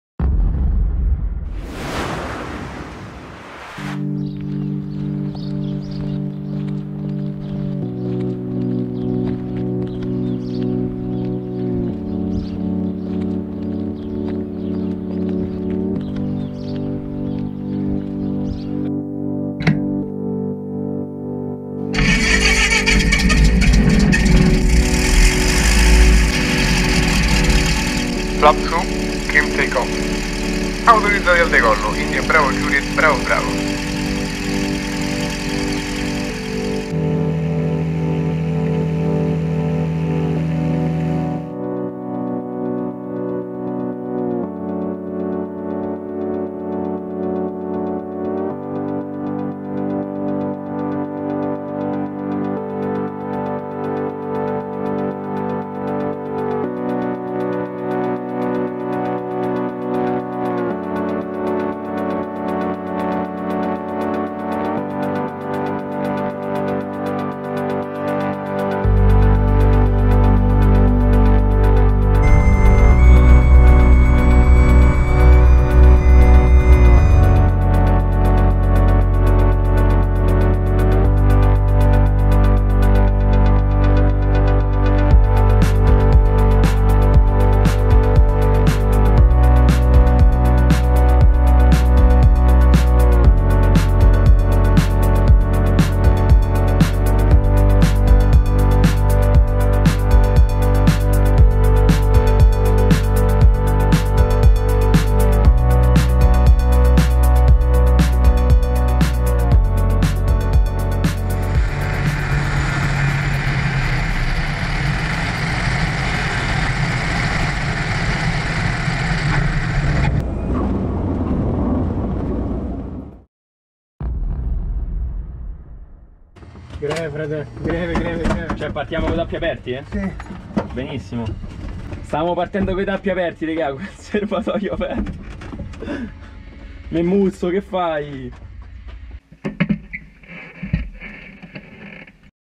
Flying Over Italy on a Cessna 🛩
Cessna 152 Ready for Take-Off! Roma - Monte Argentario

Giretto di ricognizione direzione Toscana con serbatoio aperto :)